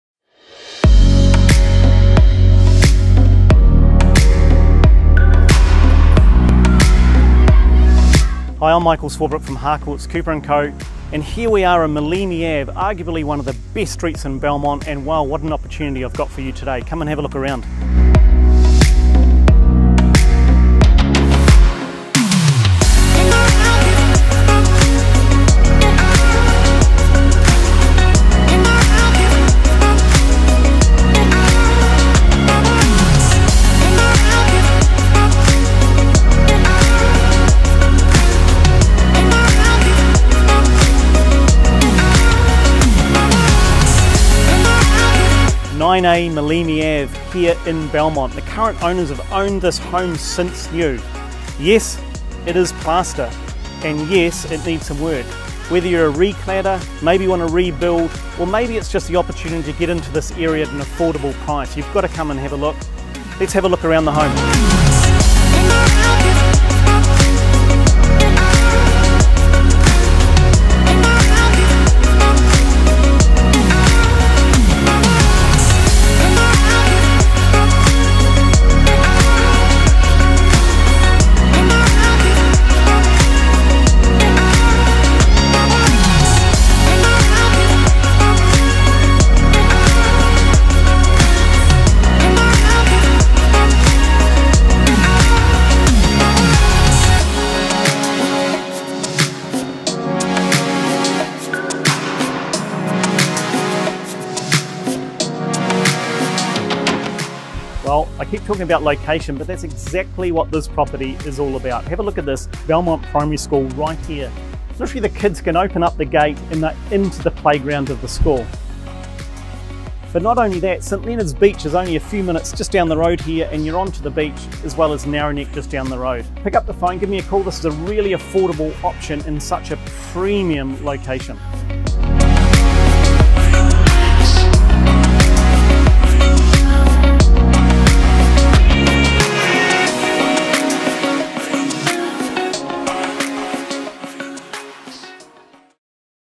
Detonate or Renovate + Location = your big chance.
9a Maleme Avenue, Belmont
Yes I am Plaster! And am being sold on an ‘as-is’ basis and that’s where your opportunity begins. Re-clad or rebuild or move in and enjoy and leave until later - the options are endless.
You can always change the house but you can’t change the location.
Maleme has long been sought after as one of those favourite quiet cul de sac’s on the eastern slopes of the peninsula. While there may be the obvious issues of a plaster build, if you’re up for the renovation you are looking at an exciting opportunity to buy into one of Belmont’s most desirable streets at a fraction of the price.